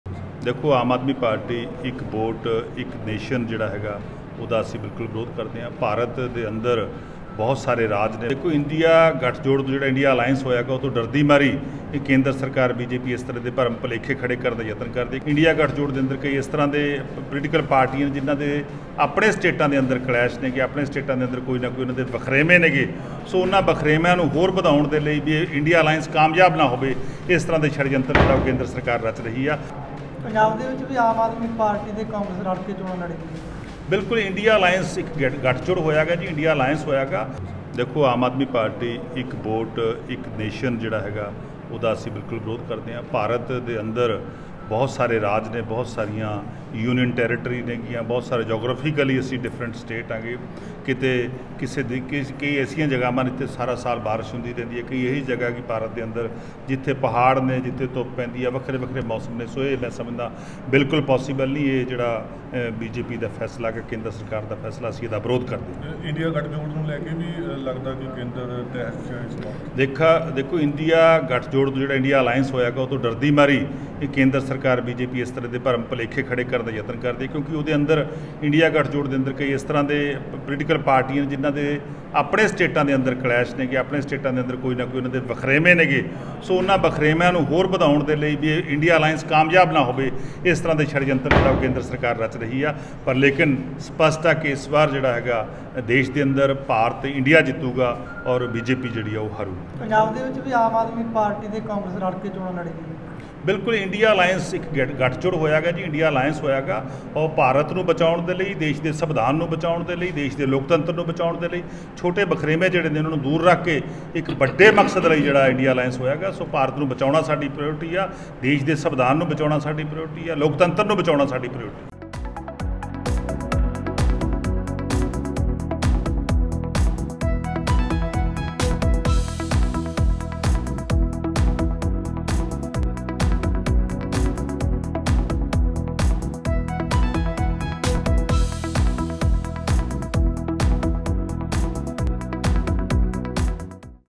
ਆਪਣੀ ਸਰਕਾਰੀ ਦੇ U-Turn 'ਤੇ ਬੋਲੇ ਮੰਤਰੀ Harpal Cheema | Punjab Tak
ਮੰਤਰੀ ਹਰਪਾਲ ਚੀਮਾ ਦੀ ਵੱਡਾ ਬਿਆਨ
'ਕਾਂਗਰਸ ਨਾਲ ਮਿਲ ਕੇ ਲੜਾਂਗੇ 2024 ਦੀਆਂ ਚੋਣਾਂ'
'ਗੱਠਜੋੜ ਤੋਂ ਡਰ ਗਈ BJP'